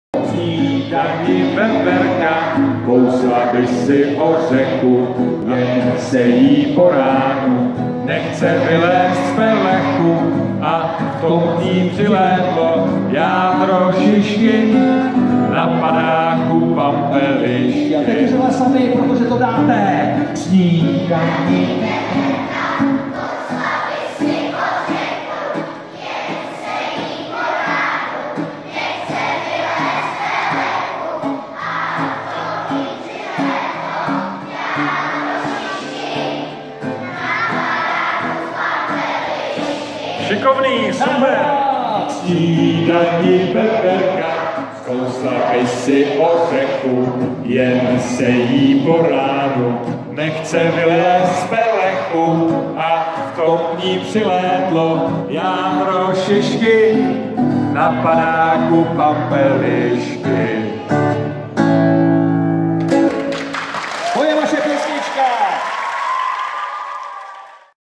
Veverka Sušice 15 listopadu 2017
Veverka

K snídani veverka kousla by si ořechu,
jen se jí po ránu nechce vylézt z pelechu.
A v tom k ní přilétlo jádro šišky
na padáku pampelišky.

Sušice – 15. listopadu 2017